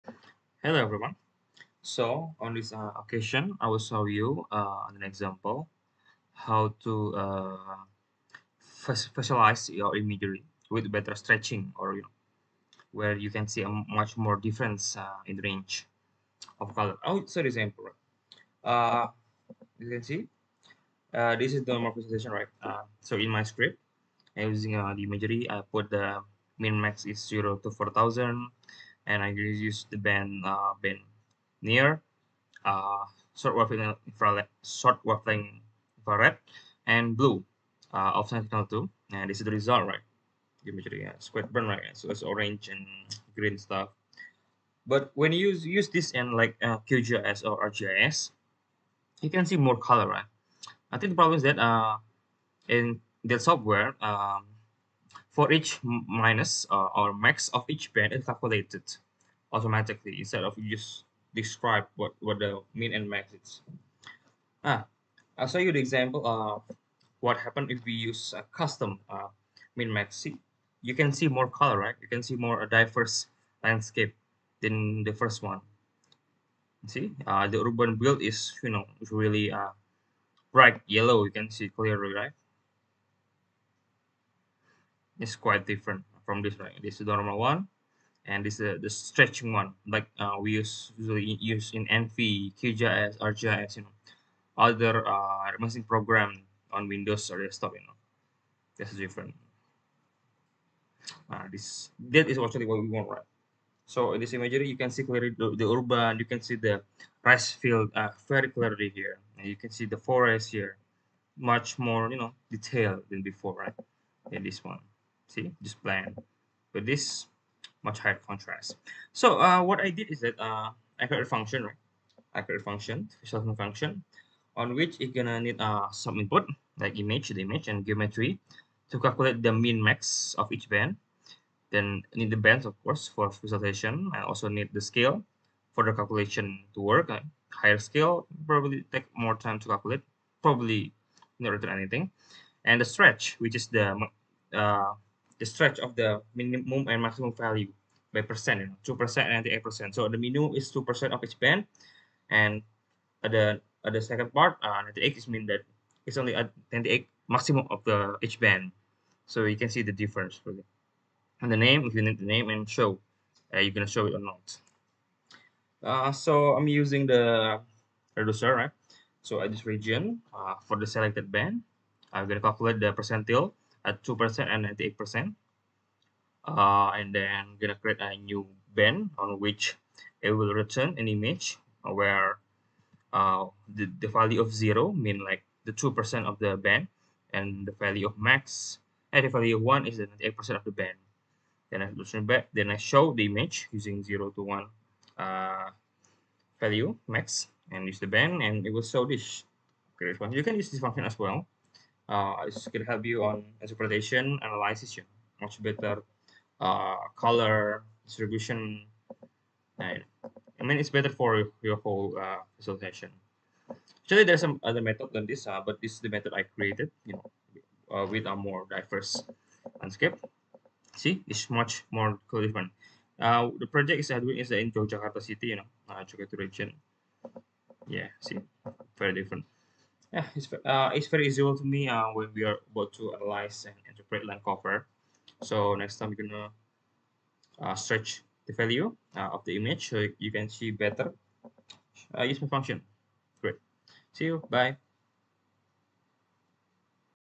Better Image Visualization with Mininum and Maximum Stretching in Google Earth Engine
Hello Geospatial Enthusiast!

Have you wondered why the imagery visualization in Google Earth Engine is quite bland? Different when using other desktops GIS & Remote Sensing programs such as QGIS, ArcGIS, ENVI, Idrisi, Erdas, etc.

Turns out, when visualizing imagery in GEE, you need to define the minimum and the maximum to show yourself. In contrast, in other software, the minimum and the maximum of each band will be calculated automatically.

In this video, I will show you how to copy that method in Google Earth Engine. We will calculate the min and max of each band that will be visualized using reduceRegion and scale the image value to that value. Then we just show the image to the map.

The motivation behind this is that with this visualization, it is easier to interpret the difference between land use and land cover with a striking contrast between classes.

If you need consultation regarding geospatial analysis and remote sensing.